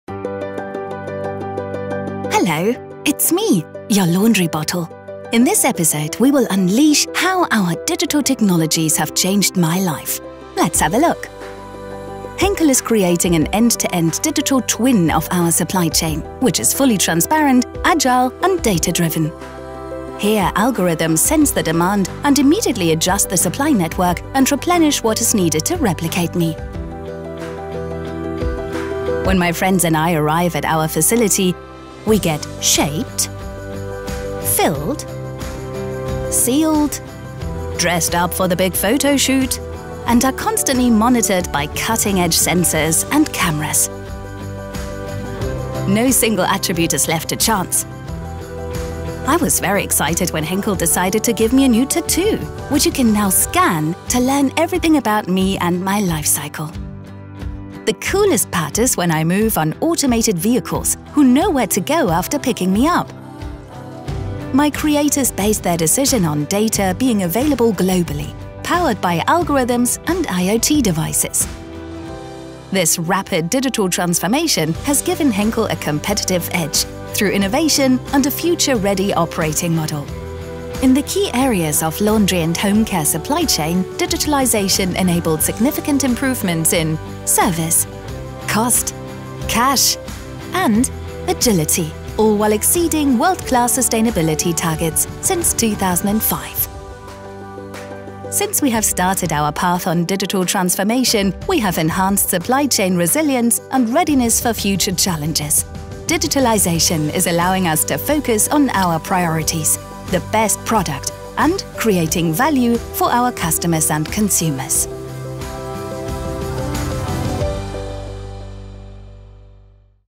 The Digital Supply Chain of Henkel's Laundry Bottle
Connecting machines and sensors, using BigData: Henkel Laundry and Home Care is digitally transforming its operations to become a totally interlinked end-to-end SupplyChain with real-time transparency on planning, production, and logistic processes. 🚀

Follow a laundry bottle its digital journey from raw and pack material receipt until customer delivery. Get to know various performance indicators that have been improved thanks to our focus on DigitalTransformation and Industry40.